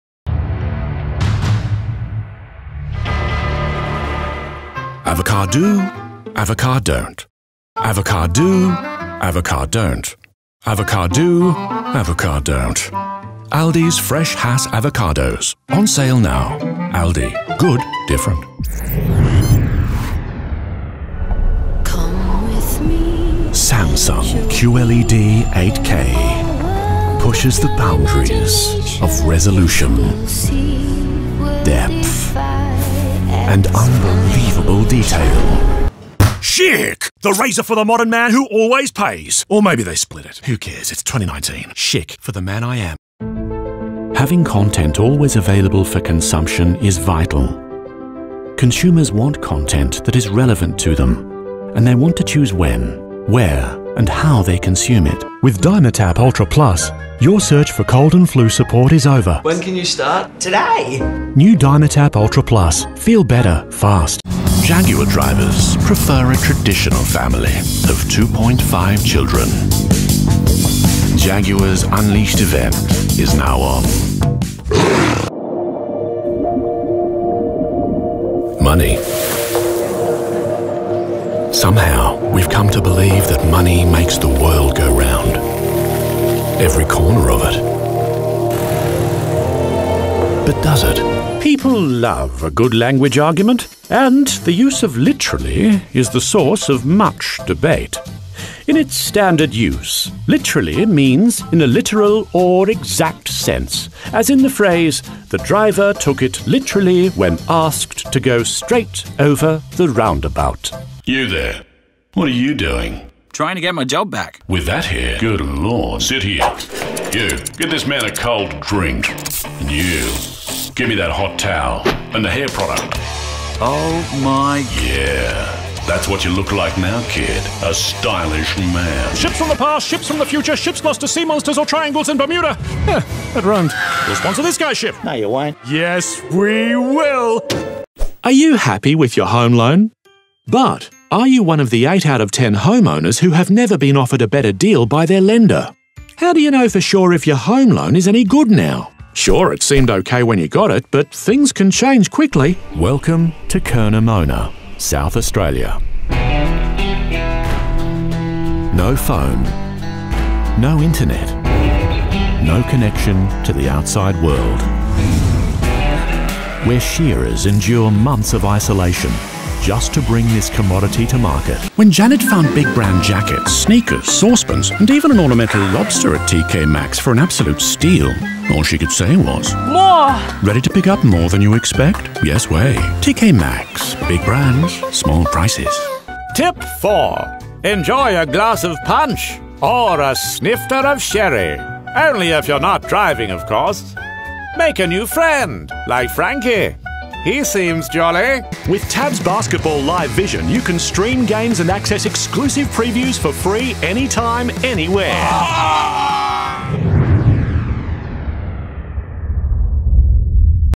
ALDI Voiceover
ALDI Australia TVC B. Produced by BMF Australia.
Voice: Colin Cassidy (UK, Oz and US accented Impressionist).